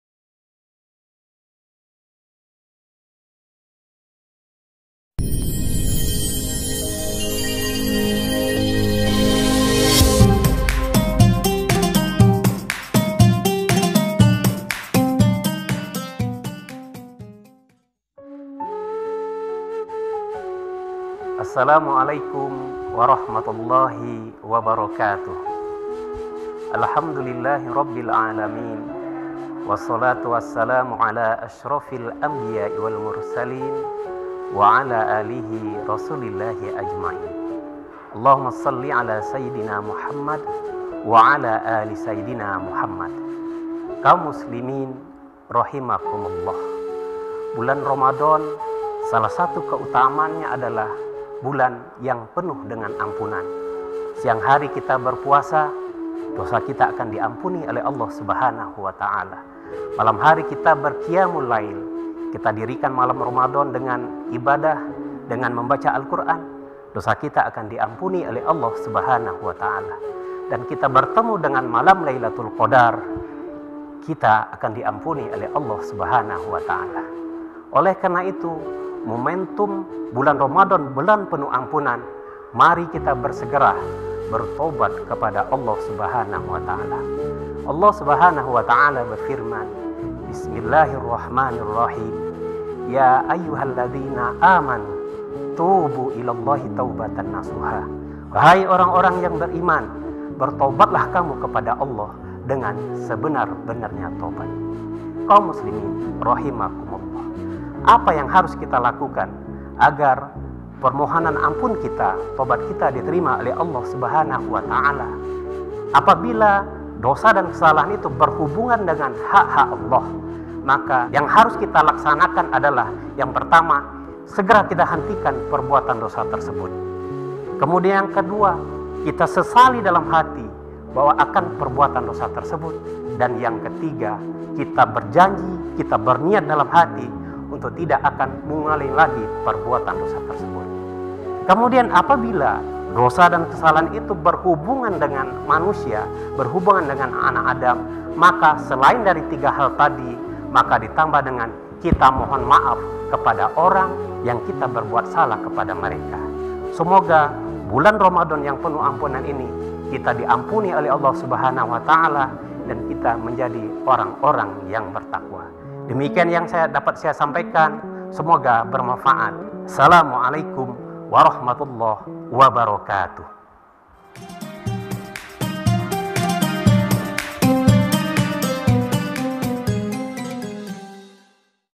Khazanah Ramadan 1442 H " Taubat Nasuha"
Assalamualaikum wr.wb

Khazanah Ramadan TNI Angkatan Udara hari ini:

“Taubat Nasuha" Yang disampaikan oleh Letkol Sus H. Zulhalifa, S.Ag.

Demikian, Wassalamualaikum wr.wb
Salam sehat
Ingat Selalu 3 M